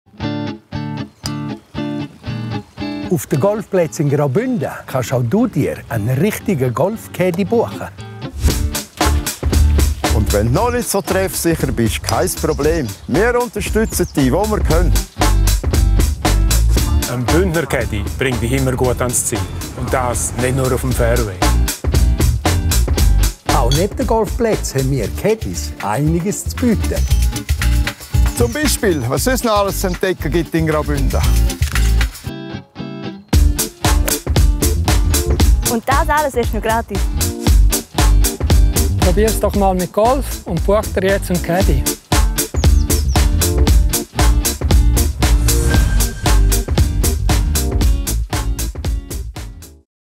Auf eine Golfrunde mit den Bündner Caddies.
Graubünden lässt auch 2022 die Caddies hochleben. 13 Bündner Caddies stehen ab der kommenden Golfsaison bereit. Lernen Sie unsere Caddies der besonderen Art in diesem Video kennen. Sie begleiten Golfer*innen auch abseits des Golfplatzes. Wie wär’s mit einer Tai-Chi-Lektion nach erfolgreichem Putten? Oder wer sagt nicht Ja zu einer Wanderung zu einem schönen Bergsee? Und wer möchte ein paar Wörter auf Rätoromanisch lernen? Die Bündner Caddies verraten Ihnen Geheimtipps in der grössten Golfdestination der Schweiz, weit über die 12 Golfplätze hinaus. Buchen Sie jetzt kostenlos Ihren Bündner Caddie.